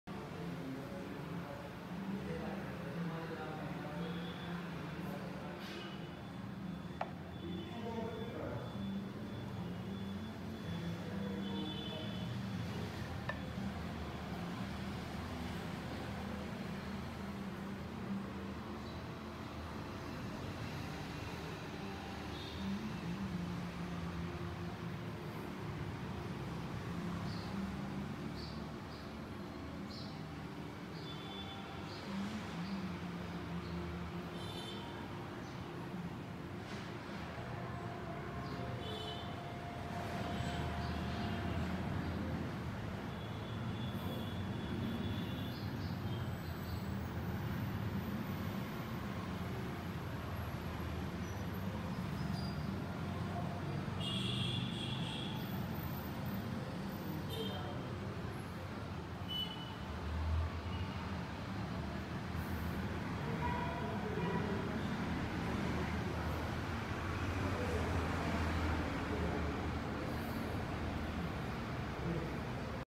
Humming Bird (Latteart)
Practicing hummingbird latteart.